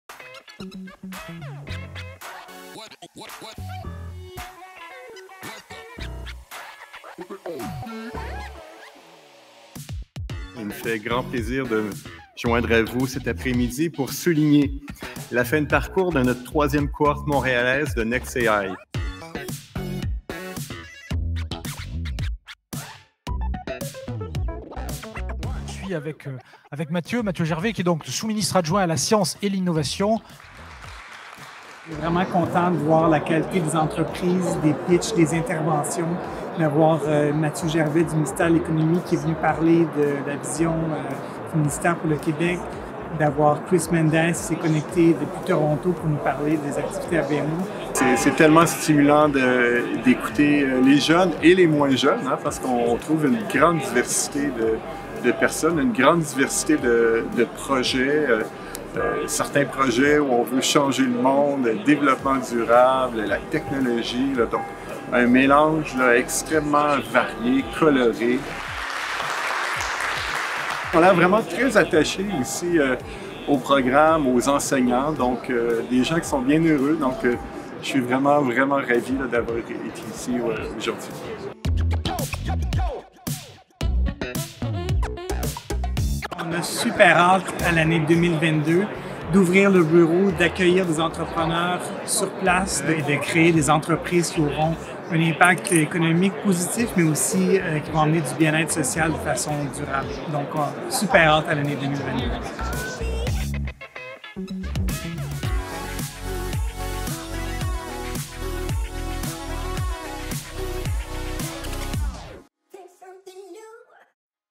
Next AI - Montréal 2021: Journée des entreprises | 2021 Venture Day
Le 28 Septembre 2021 avait lieu la Journée des entreprises de Next AI - Montréal 2021. Lors de cet événement hybride, à la fois virtuel et en présentiel, la cinquantaine d'entrepreneur-e-s du programme ont pu partager leurs idées, leurs modèles d'affaires et leurs prototypes.
On September 28, 2021 was held the Next AI - Montreal 2021 Corporate Day. During this hybrid event — both virtual and in person — the 50 or so entrepreneurs of the program were able to share their ideas, business models and prototypes.